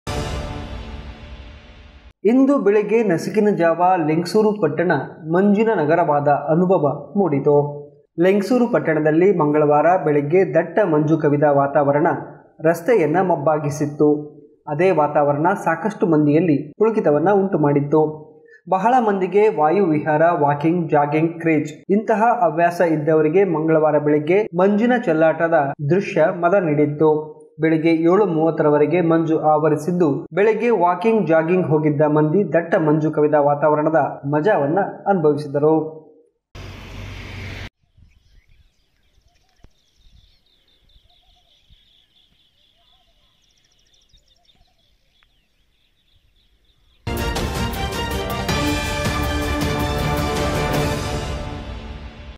ದಟ್ಟ ಮಂಜು ಕವಿದ ವಾತಾವರಣ ಮಂಜಿನ ನಗರವಾದ ಲಿಂಗಸುಗೂರು Lingasugur News SLVC NEWS 02 11 2021
SLVCChannel